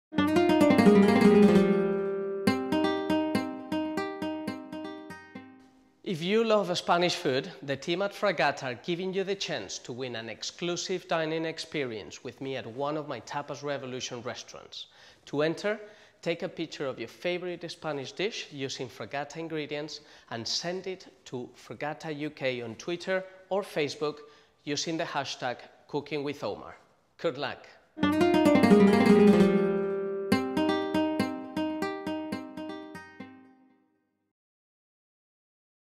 Win An Exclusive Dinner with Omar Allibhoy from Tapas Revolution!
Fragata UK and Omar Allibhoy from Tapas Revolution have teamed up to bring you a once in a lifetime chance to win an exclusive dinner with Omar Allibhoy at one of his Tapas Revolution restaurants in London. Head to our Facebook and Twitter pages to learn more!